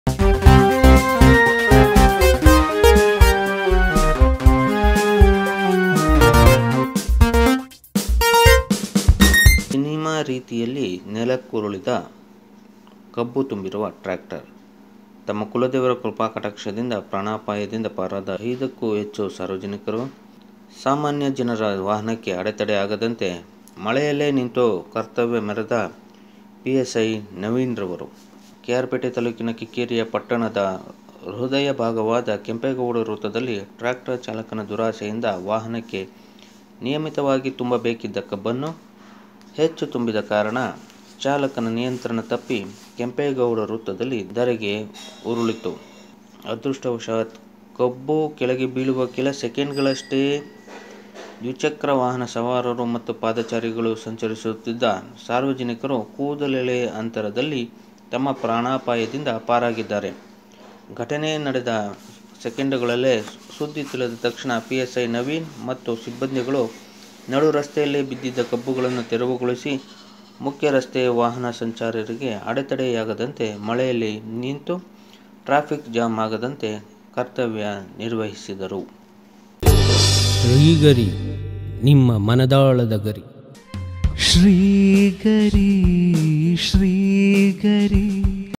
ಸಿನಿಮಾ ರೀತಿಯಲ್ಲಿ ನೆಲಕ್ಕುರುಳಿದ ಕಬ್ಬು ತುಂಬಿರುವ ಟ್ಯಾಕ್ಟರ್
ಸಿನಿಮಾ ರೀತಿಯಲ್ಲಿ ನೆಲಕ್ಕುರುಳಿದ ಕಬ್ಬು ತುಂಬಿರುವ ಟ್ಯಾಕ್ಟರ್ ತಮ್ಮ ಕುಲದೇವರ ಕೃಪಾಕಟಾಕ್ಷದಿಂದ ಪ್ರಾಣಾಪಾಯದಿಂದ ಪಾರಾದ 5 ಹೆಚ್ಚು ಸಾರ್ವಜನಿಕರು. ಸಾಮಾನ್ಯ ಜನರ ವಾಹನಕ್ಕೆ ಅಡತಡೆ ಆಗದಂತೆ ಮಳೆಯಲ್ಲೇ ನಿಂತು ಕರ್ತವ್ಯ ಮೆರೆದ ಪಿಎಸ್ಐ ನವೀನ್ ರವರು*

ಕೆ ಆರ್ ಪೇಟೆ ತಾಲೂಕಿನ ಕಿಕ್ಕೇರಿಯ ಪಟ್ಟಣದ ಹೃದಯಭಾಗವಾದ ಕೆಂಪೇಗೌಡ ವೃತ್ತದಲ್ಲಿ ಟ್ರ್ಯಾಕ್ಟರ್ ಚಾಲಕನ ದುರಾಸೆಯಿಂದ ವಾಹನಕ್ಕೆ ನಿಯಮಿತವಾಗಿ  ತುಂಬ ಬೇಕಿದ್ದ ಕಬ್ಬನ್ನು ಹೆಚ್ಚು ತುಂಬಿದ ಕಾರಣ ಚಾಲಕನ ನಿಯಂತ್ರಣ ತಪ್ಪಿ ಕೆಂಪೇಗೌಡ ವೃತ್ತದಲ್ಲಿ ಧರೆಗೆ ಉಳಿದಿದೆ ಅದೃಷ್ಟವಶಾತ್ ಕಬ್ಬು ಕೆಳಗೆ ಬೀಳುವ ಕೆಲಸ ಸೆಕೆಂಡ್ ಗಳಷ್ಟೇ ದ್ವಿಚಕ್ರ ವಾಹನ ಸವಾರರು ಮತ್ತು ಪಾದಚಾರಿಗಳು ಸಂಚರಿಸುತ್ತಿದ್ದ ಸಾರ್ವಜನಿಕರು ಕೂದಲ ಹಂತರದಲ್ಲಿ ತಮ್ಮ ಪ್ರಾಣಾಪಾಯದಿಂದ ಪಾರಾಗಿದ್ದಾರೆ..

ಘಟನೆ ನಡೆದ ಸೆಕೆಂಡುಗಳಲ್ಲೇ ಸುದ್ದಿ ತಿಳಿದ ತಕ್ಷಣ ಪಿಎಸ್ಐ ನವೀನ್ ಮತ್ತು ಸಿಬ್ಬಂದಿಗಳು ನಡುರಸ್ತೆಯಲ್ಲೇ ಬಿದ್ದಿದ್ದ ಕಬ್ಬುಗಳನ್ನು ತೆರವುಗೊಳಿಸಿ ಮುಖ್ಯರಸ್ತೆ ವಾಹನ ಸಂಚಾರರಿಗೆ ಅಡೆತಡೆ ಯಾಗದಂತೆ ಮಳೆಯಲ್ಲಿ ನಿಂತು ಟ್ರಾಫಿಕ್ ಜಾಮ್ ಆಗದಂತೆ ಕರ್ತವ್ಯ ನಿರ್ವಹಿಸಿದರು.